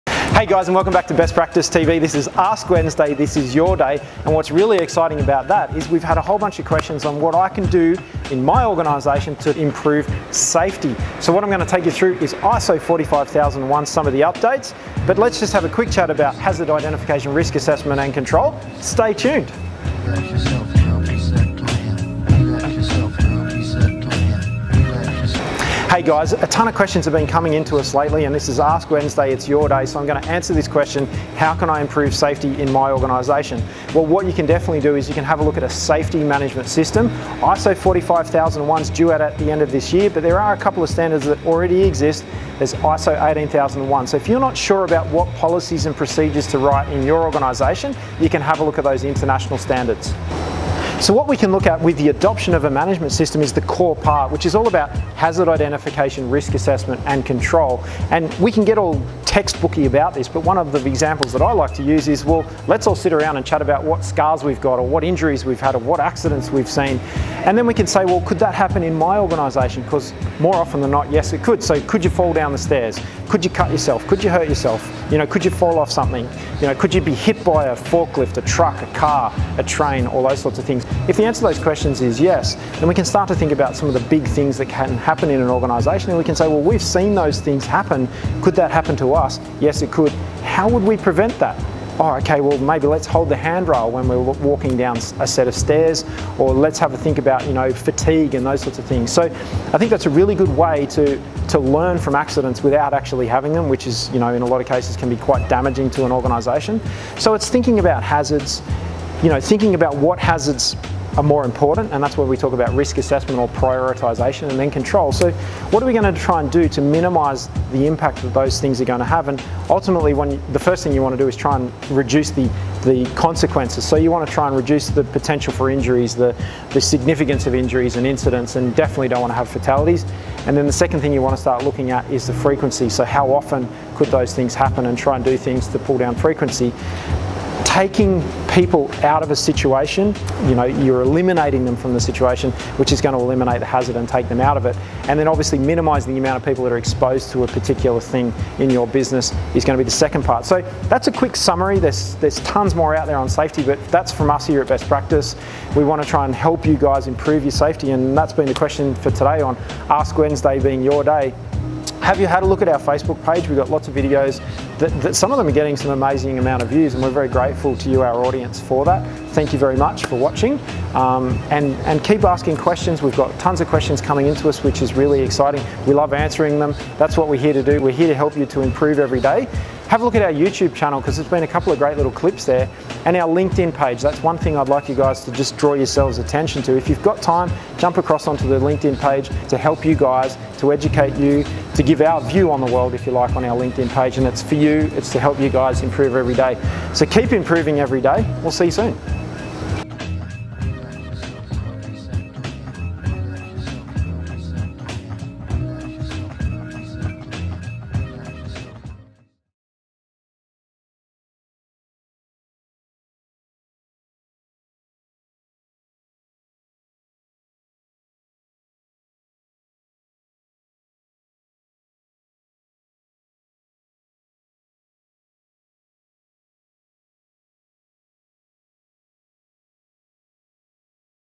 AskWednesday Talks Safety! Risk Assessment, Hazard Identification and More!
It's Wednesday! That means it's time for Kobi to answer questions from our subscriber base... This week we've received a few questions in regard to risk assessment and hazard identification in business, so sit back and hear Kobi's take on all things safety-related in the context of your business.